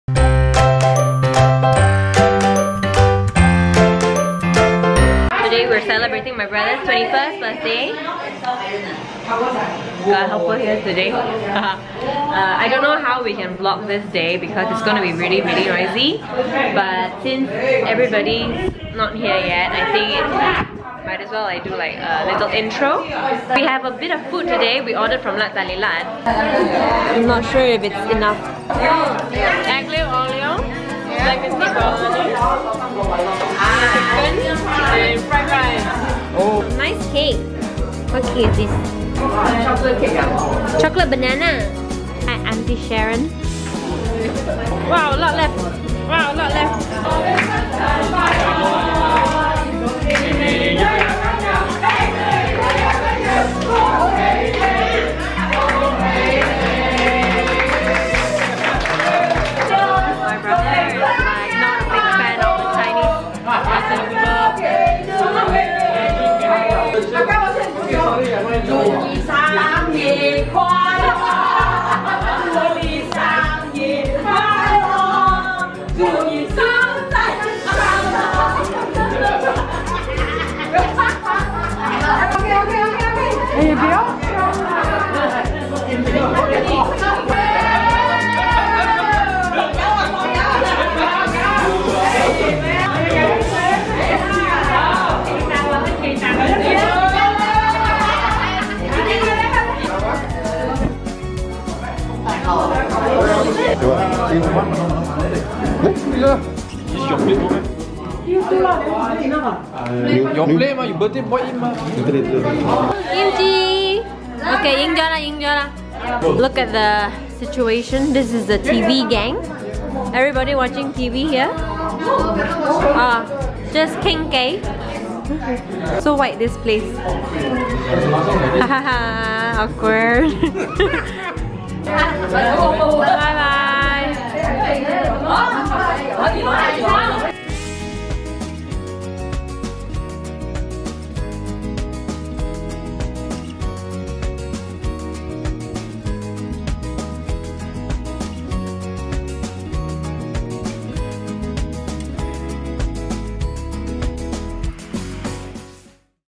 HIS FAVORITE PARTY EVER (??)
THUMBS UP if you liked the video x

MY FIRST VLOG FROM 2018!! Celebrating Nick's 21st as his birthday is pretty much earlier in the year. Let's just say he isn't a big fan of parties. You'll probably see why. This is an especially shorter vlog than the usuals as it wasn't possible to put this together with the rest of the vlogs.

Catering for the party was by Lat Tali Lat cafe - and it was SAH GOOD.